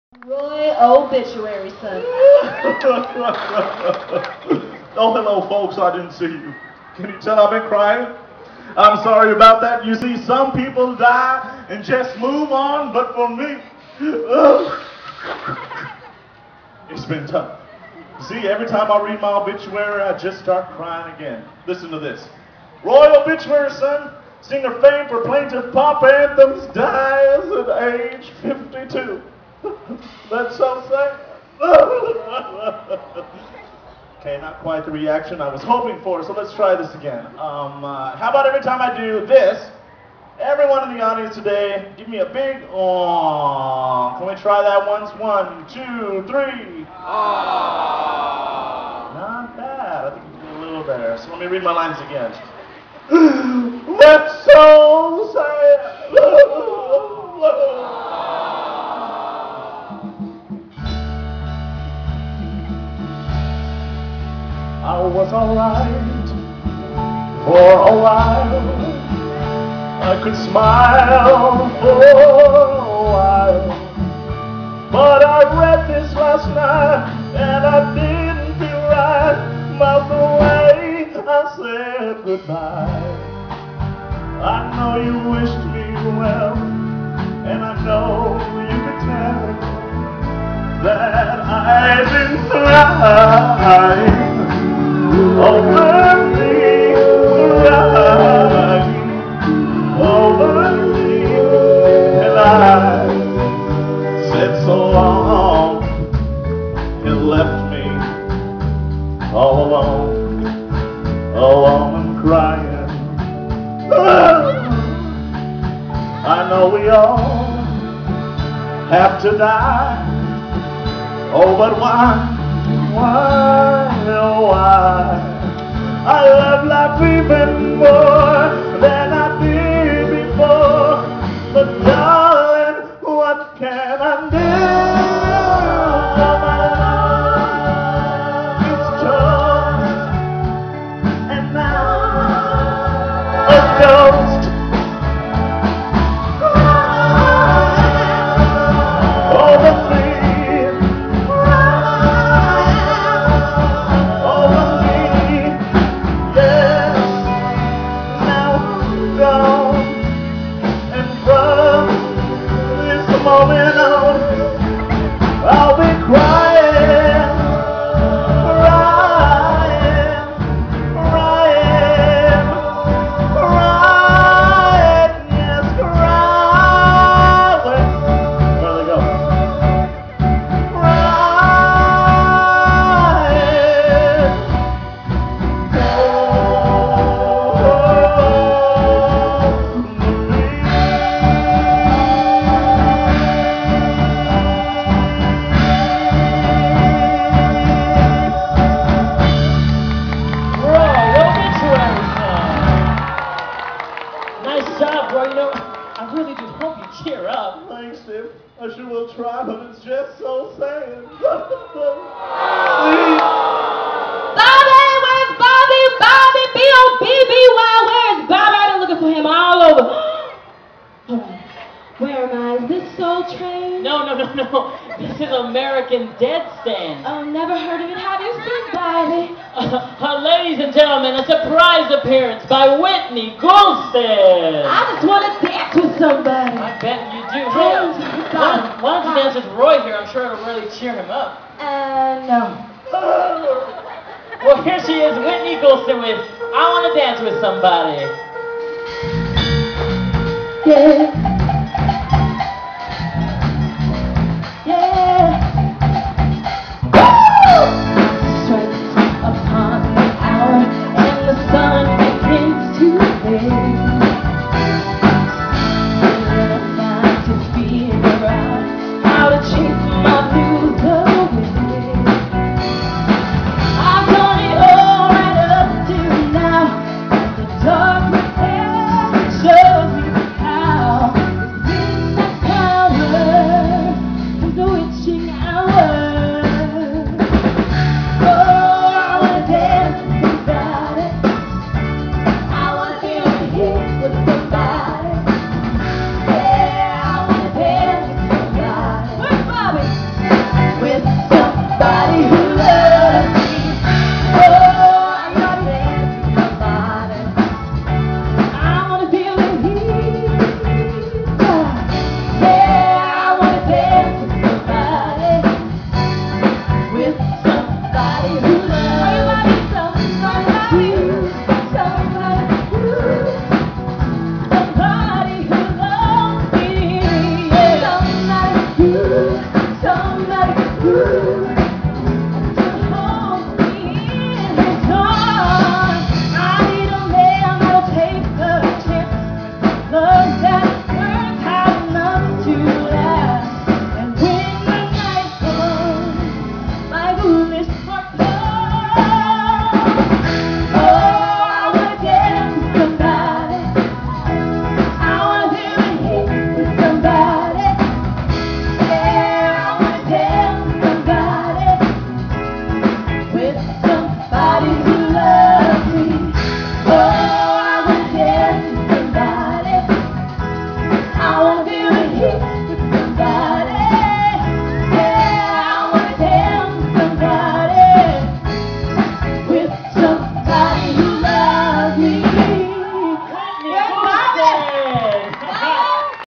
Last Chance Legends 2012 - Roy Orbison - Whitney Houston
This is Last Chance Legends at Six Flags Fiesta Texas during Fright Fest 2012...